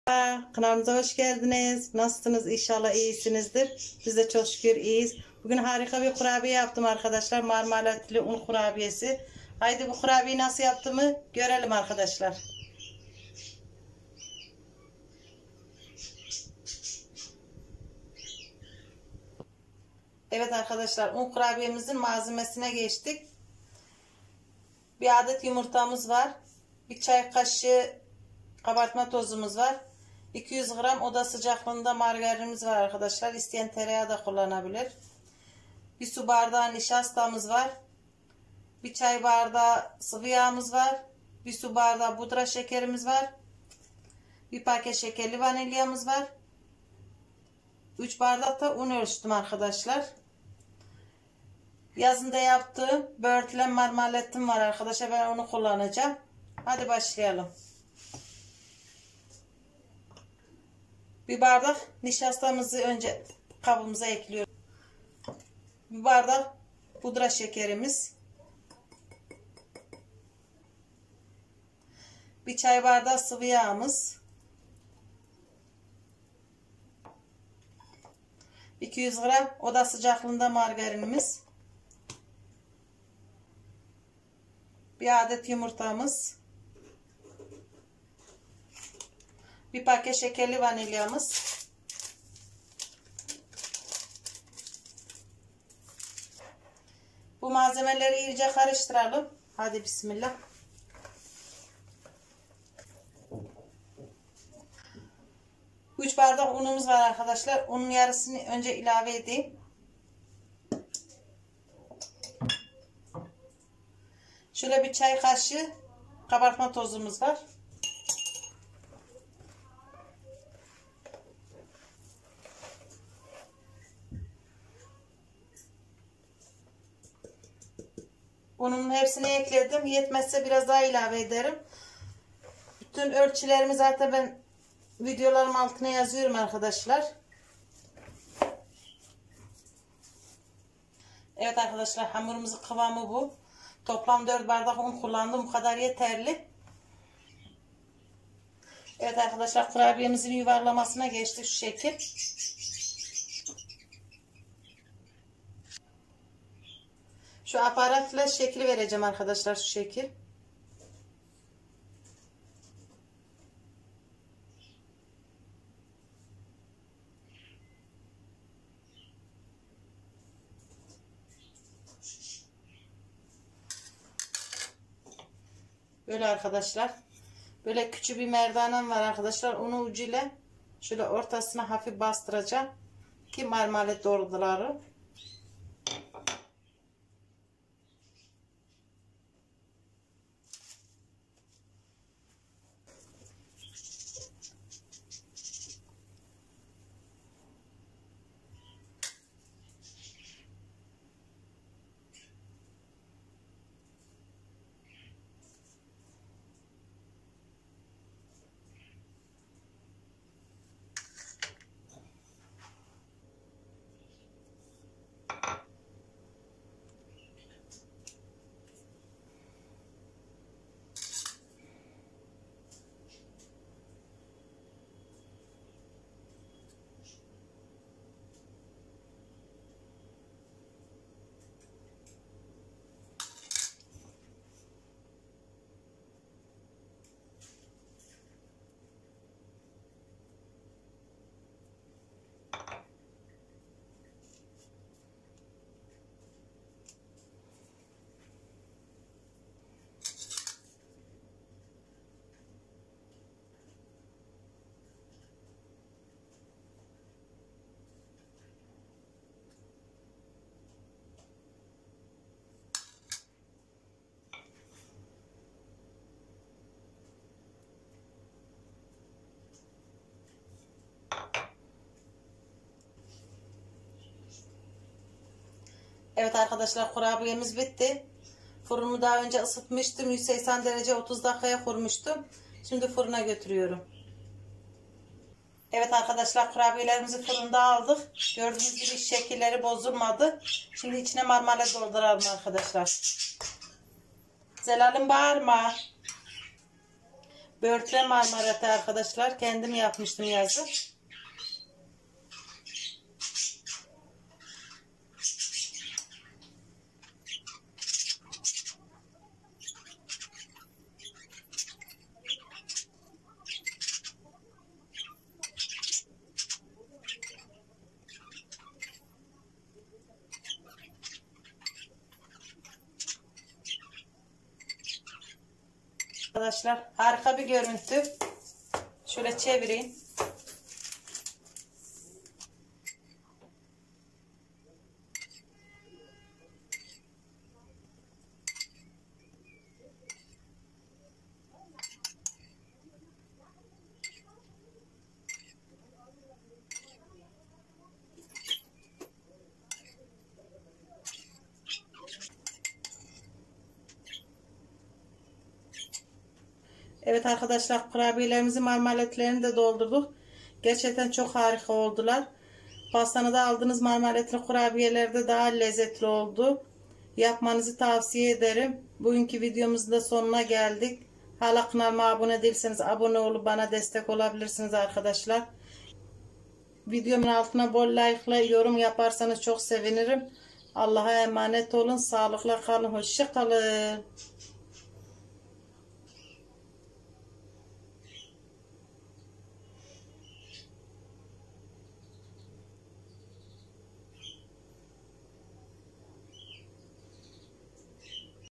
Marmelatlı Un Kurabiyesi
Tarif :

1 su bardağı nişasta
1 su bardağı pudra şekeri
1 çay bardağı sıvı yağ
200 gr margarin veya tereyağı
1 adet yumurta
1 paket şekerli vanilya
1 çay kaşığı kabartma tozu
4 bardak un
3 yemek kaşığı marmalet